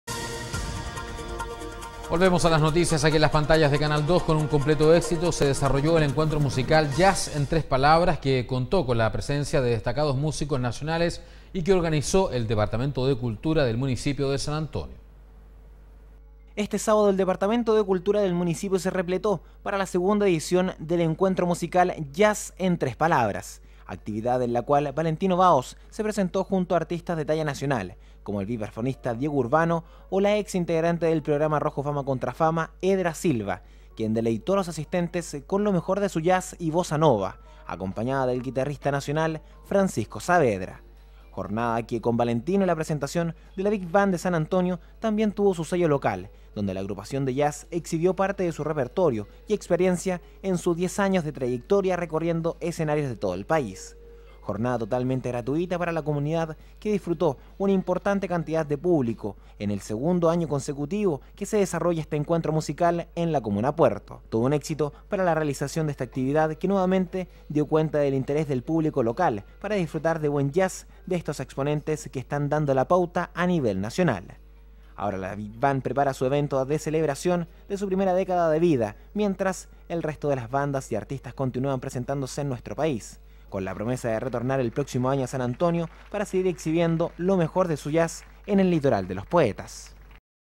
30 JUL 2013 CICLO DE JAZZ
Con un completo éxito se desarrolló el encuentro musical Jazz en tres palabras, que contó con la presencia de destacados músicos nacionales y que organizó el departamento de cultura del municipio de San Antonio.